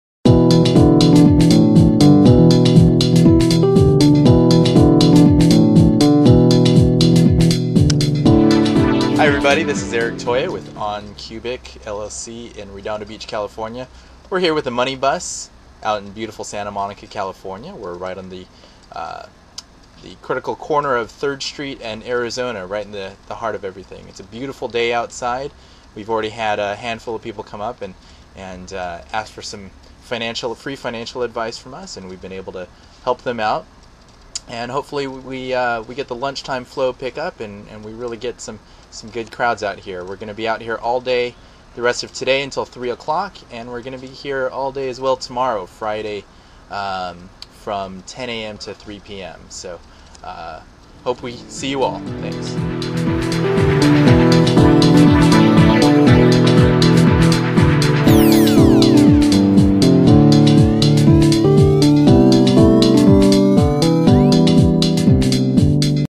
Your Money Bus Eric Toya LA, CA
The Your Money Bus tour was in Los Angeles, CA on March 12, 2009 to provide free financial advice to residents.  Eric Toya provides his thoughts on the tour and the world of finance.